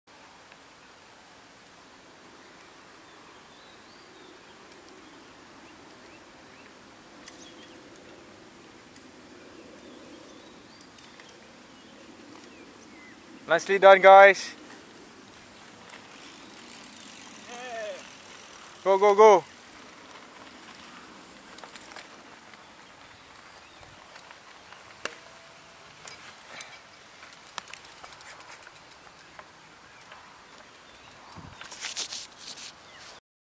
Xterra Malaysia World Championship Bike Course Preview Part 5
The part where you may get confused seeing riders coming back. Only happen if you do not read the Race Director's instruction or follow signage.